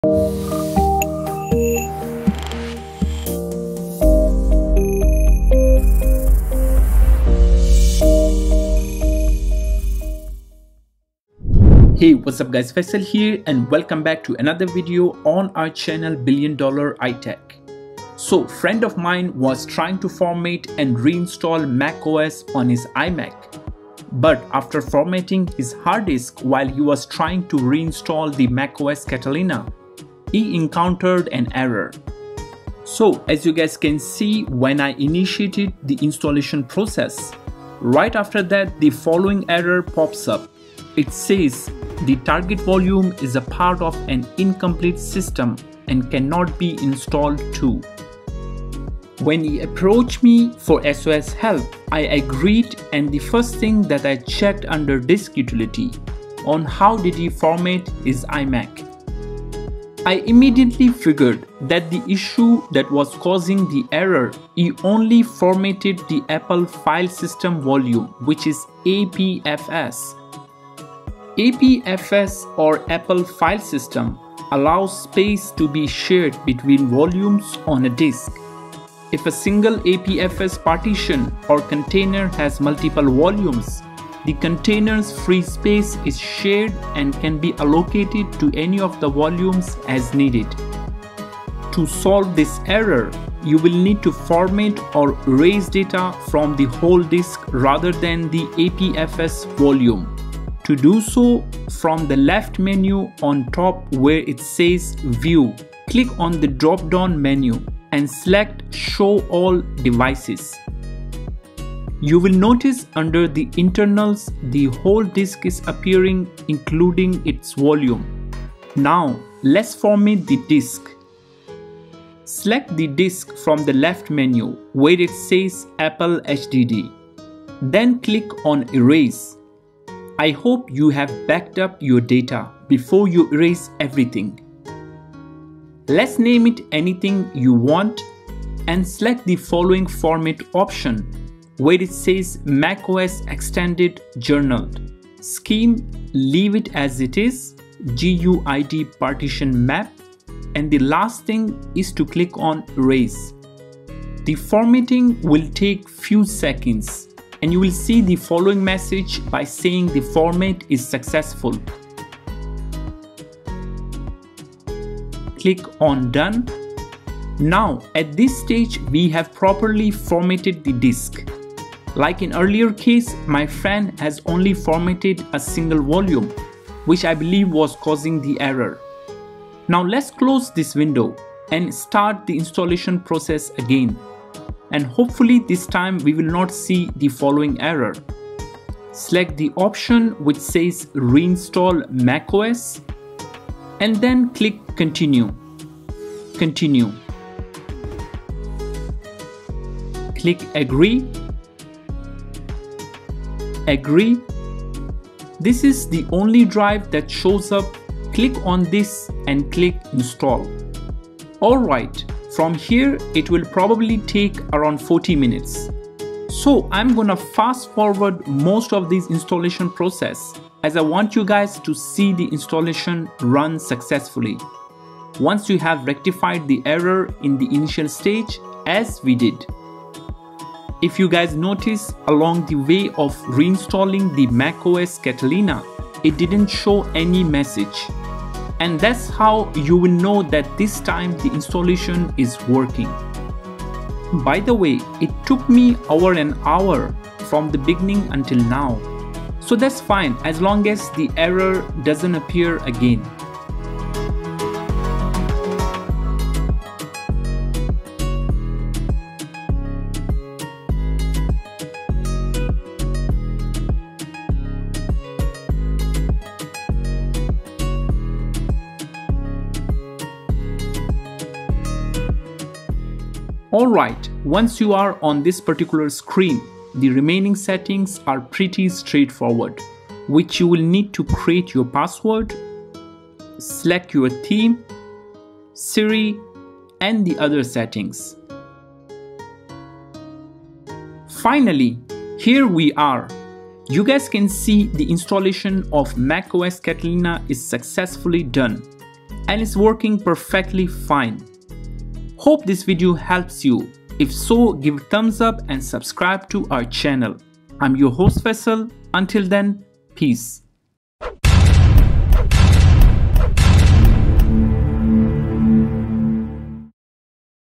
macOS | The Target Volume is Part of an Incomplete System and Cannot be Installed to.
The Target Volume is Part of an Incomplete System and Cannot be Installed to.
It is one of the common errors when you plan to reinstall your macOS for some reason and if you have not properly format the disk this error appears.